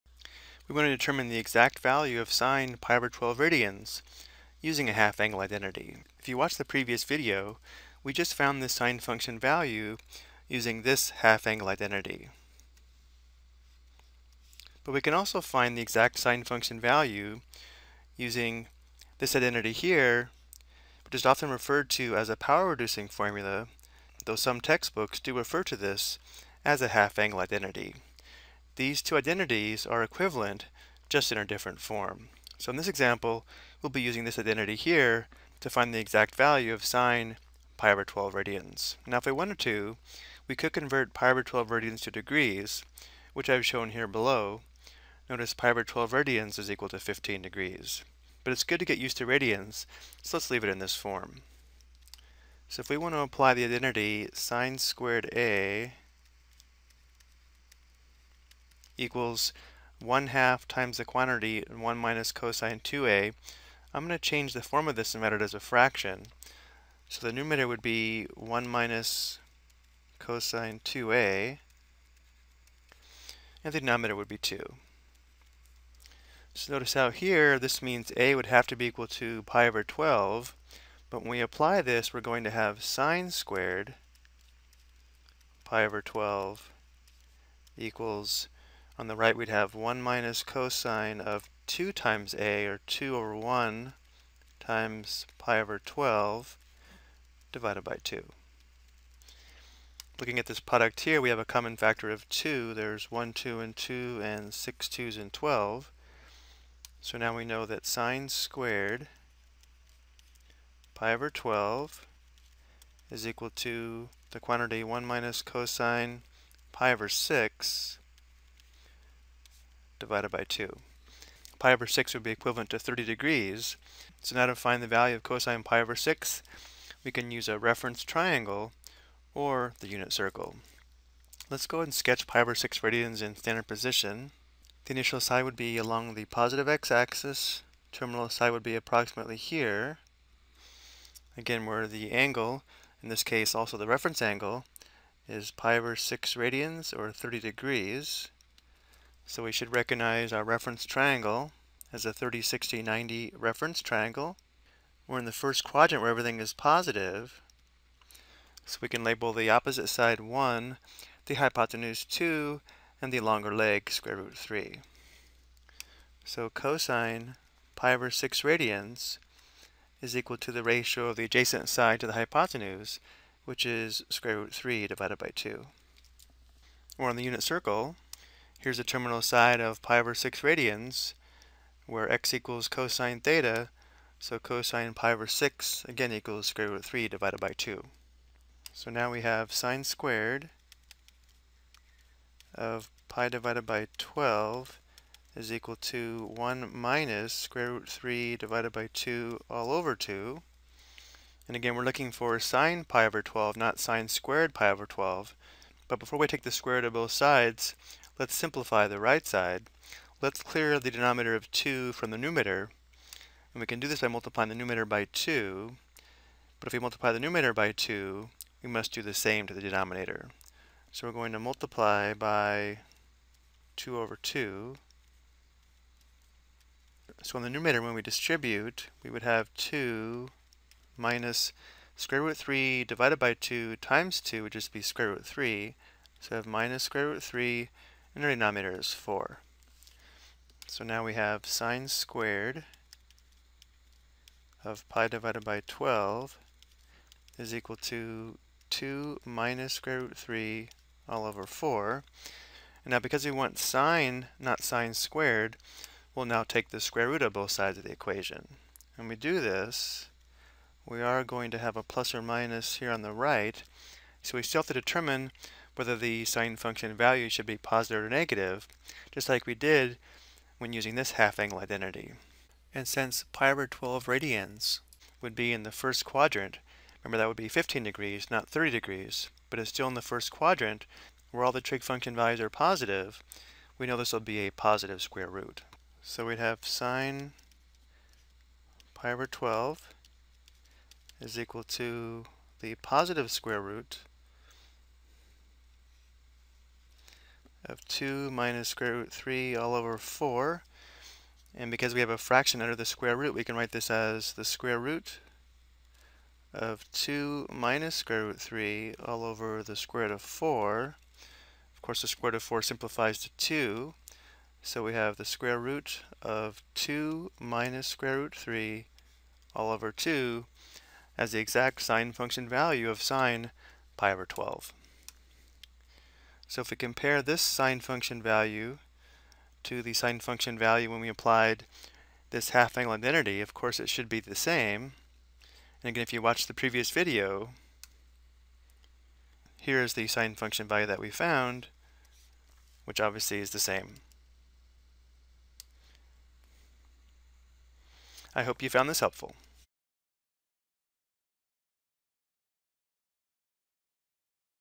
Ex: Determine a Sine Function Value Using a Alternative Half Angle Identity (Radians)
This video provides an example of how to determine the exact value of sin(pi/12) using a half angle identity using an alternative half angle identity.